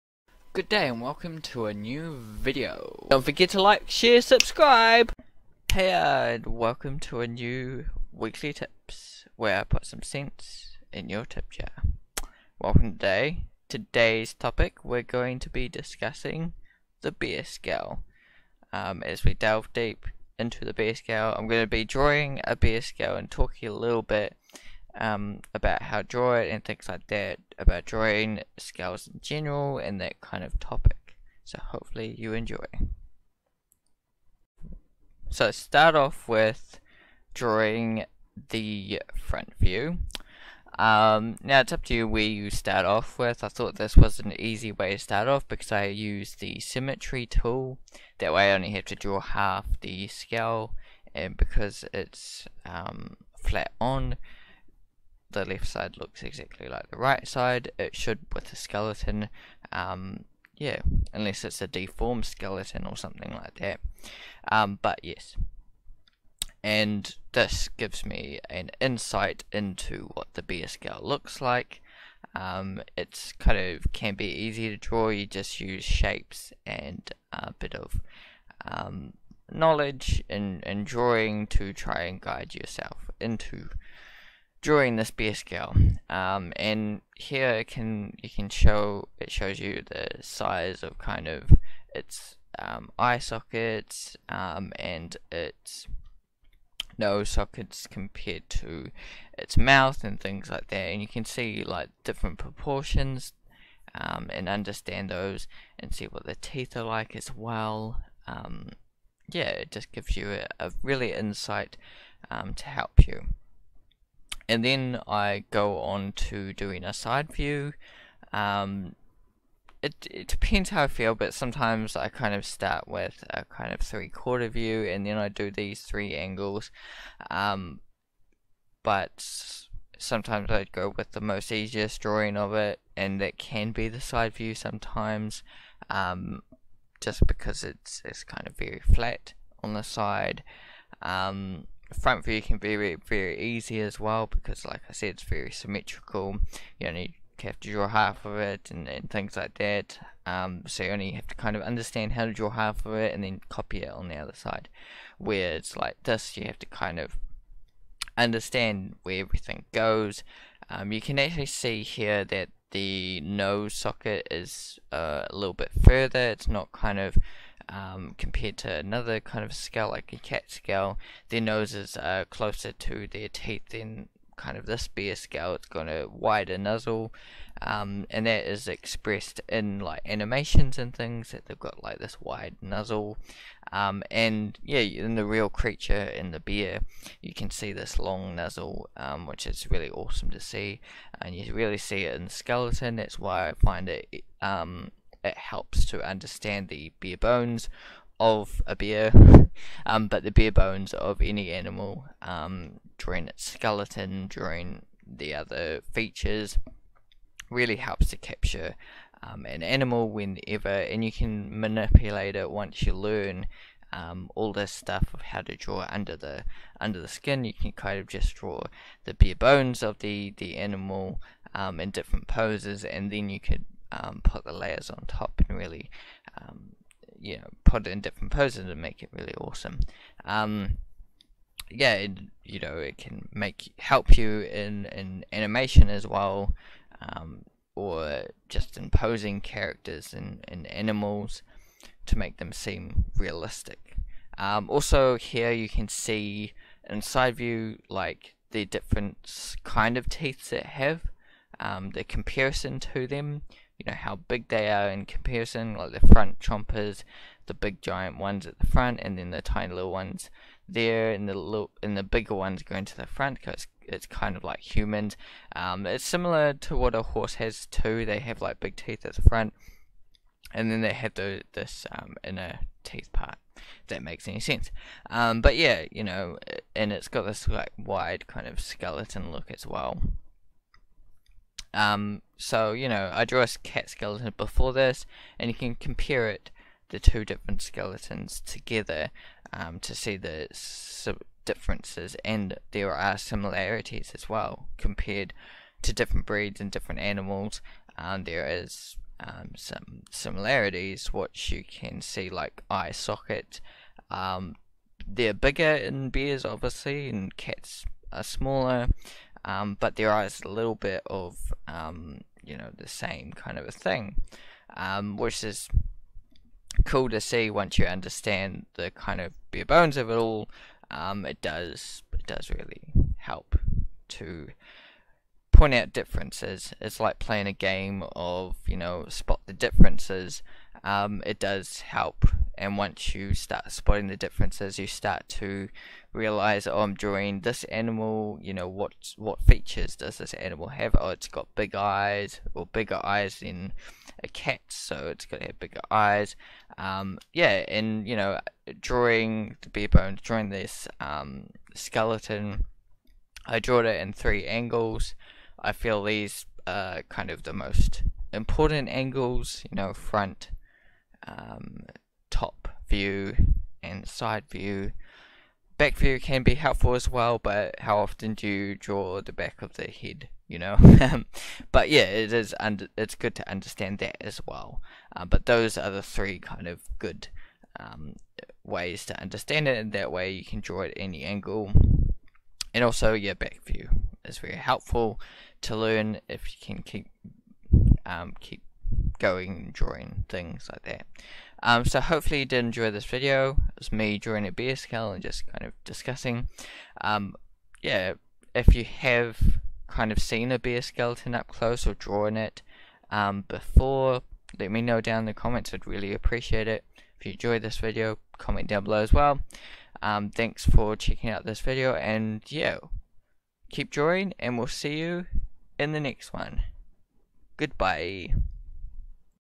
The Bear bones of the skull
Heya And welcome to weekly tips. In this video I discuss about drawing the bear skull. You can see me draw the bear skull in different angles as I point out some tips.